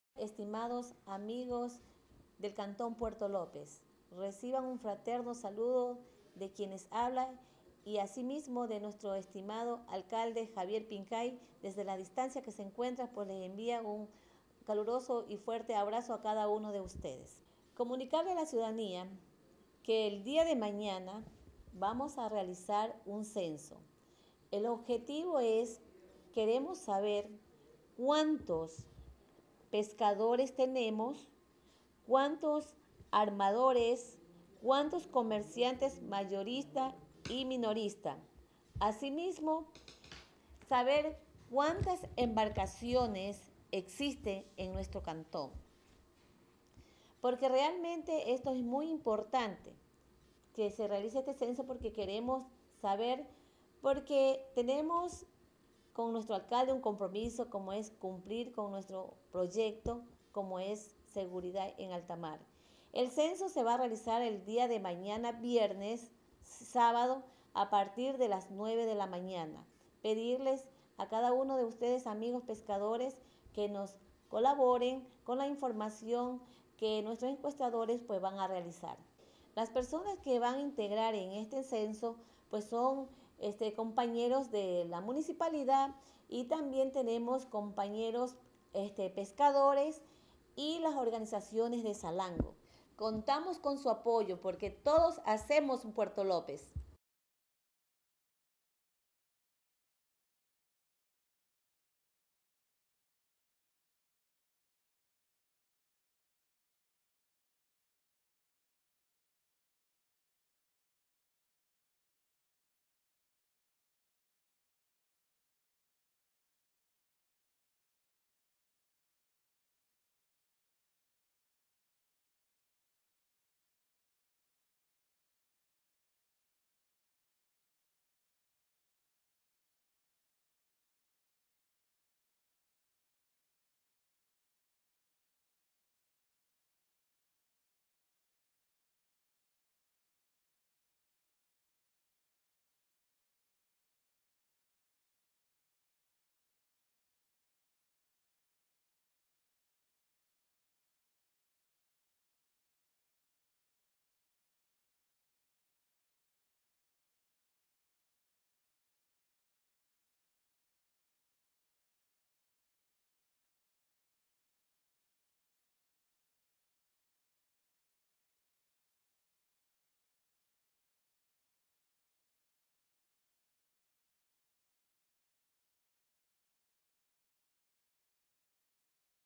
Digna Quijije Mero - Alcaldesa (S) Gad-Puerto López
Gad-Cantón Puerto López realizará Censo Pesquero
Información que fortalecerá al sector pesquero y al Plan de Desarrollo y Ordenamiento Territorial.